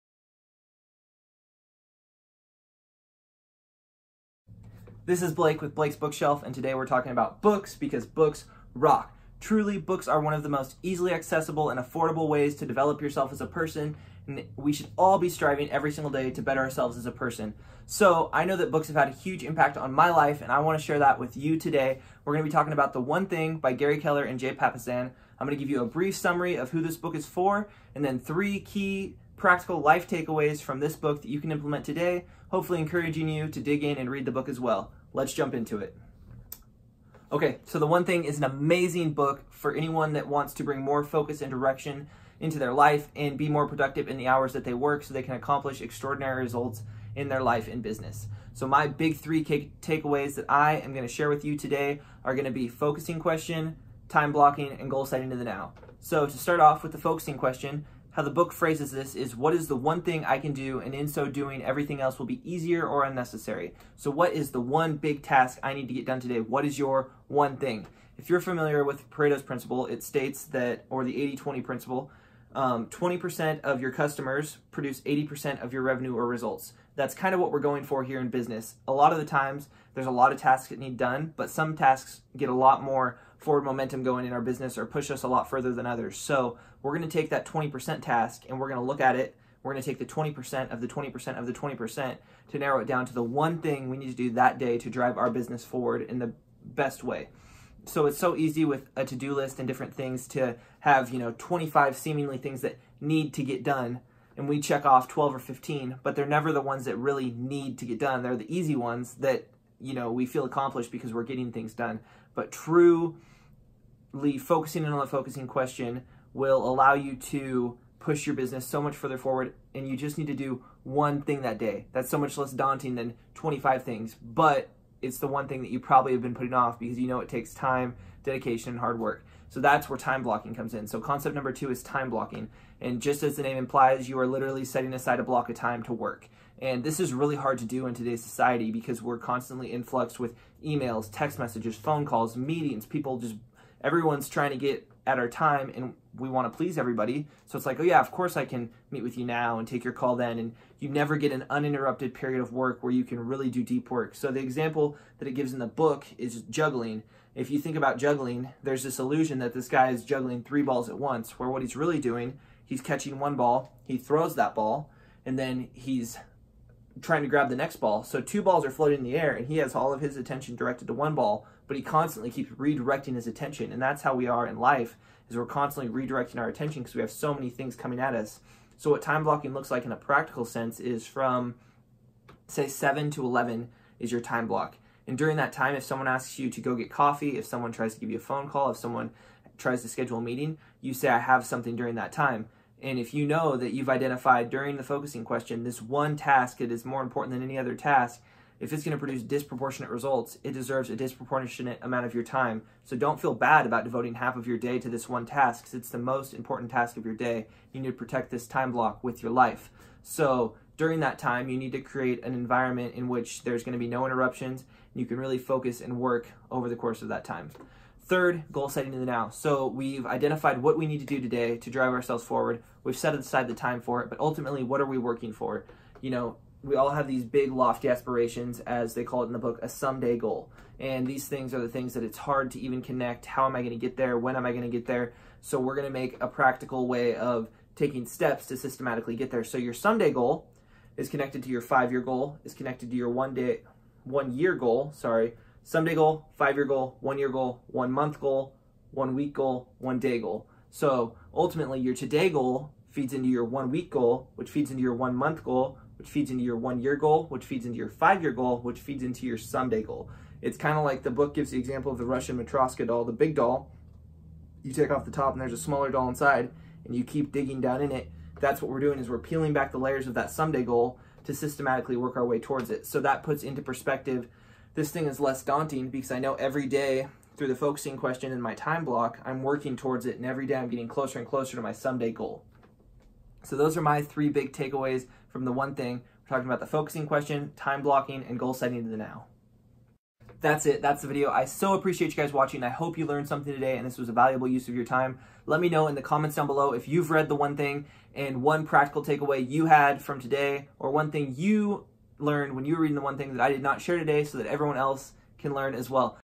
The One Thing - Summary + Key Points
The One Thing by Gary Keller and Jay Papasan
A great read to bring focus and productivity into your life.

The three concepts that most impacted me in this book were:
1.) The Focusing Question
2.) Time Blocking
3.) Goal Setting to the Now